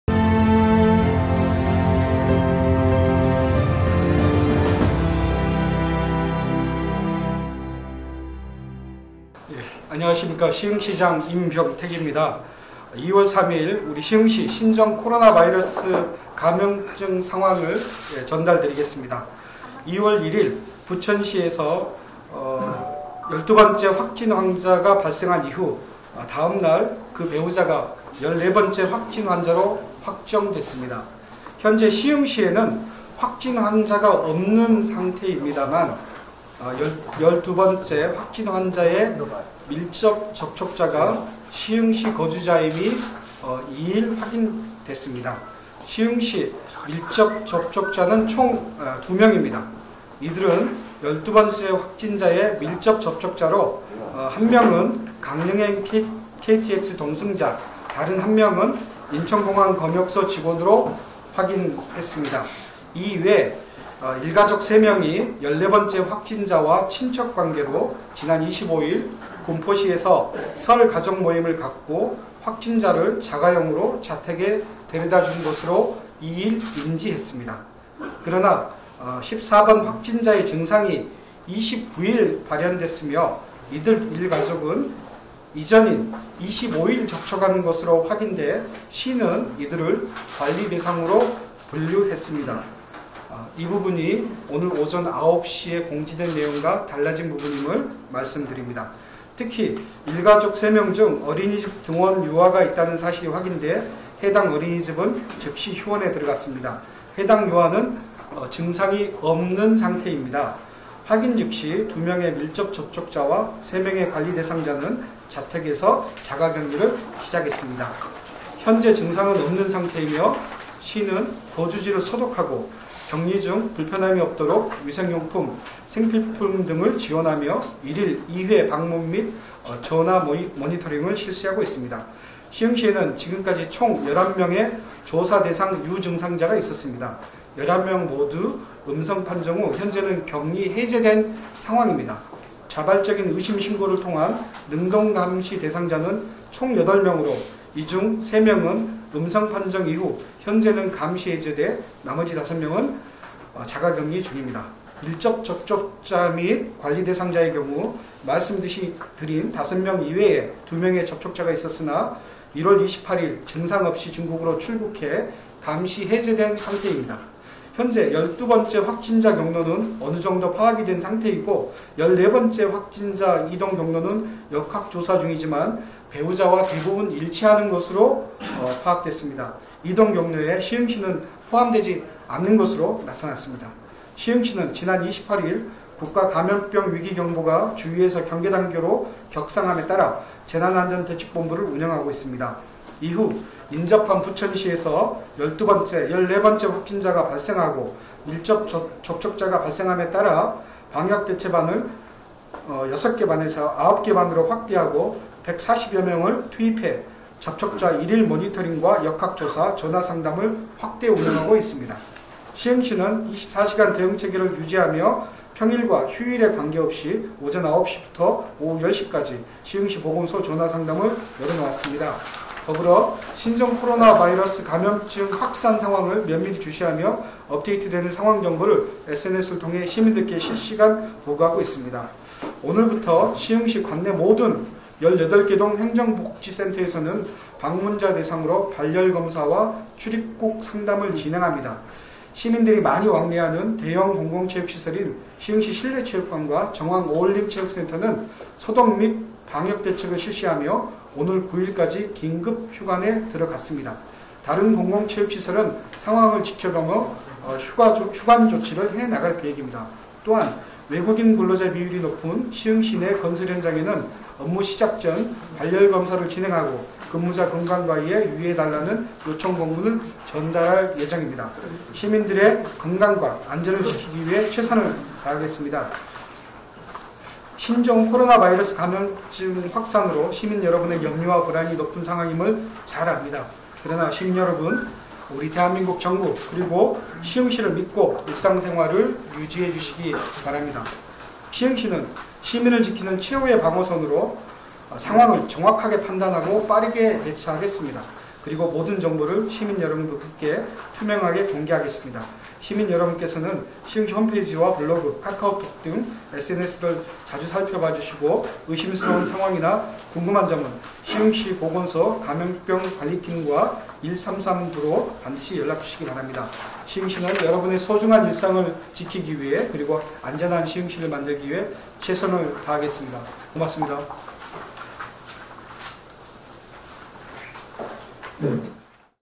[언론브리핑]시흥시 신종 코로나바이러스 감염증 상황 전달
2월 1일 부천시에서 12번째 확진 환자가 발생한 이후, 다음날 그 배우자가 14번째 확진 환자로 확정됐습니다.
12번째, 확진 환자의 밀접접촉자가 시흥시 거주자임을 2일 확인했습니다.

시흥시 밀접접촉자는 총 2명입니다.
이들은 12번째 확진자의 밀접접촉자로, 1명은 강릉행 KTX 동승자, 다른 1명은 인천공항 검역소 직원으로 확인했습니다.

이 외 일가족 3명이 14번째 확진자와 친척 관계로 지난 25일 군포시에서 설 가족 모임을 갖고 확진자를 자가용으로 자택에 데려다준 것으로 2일 인지했습니다.

그러나 14번 확진자의 증상이 29일 발현됐으며 이들 일가족은 이 전인 25일 접촉한 것으로 확인돼 시는 이들을 관리 대상으로 분류했습니다.

특히 일가족 3명 중 어린이집 등원 유아가 있다는 사실이 확인돼 해당 어린이집은 즉시 휴원에 들어갔습니다.
해당 유아는 증상이 없는 상태입니다.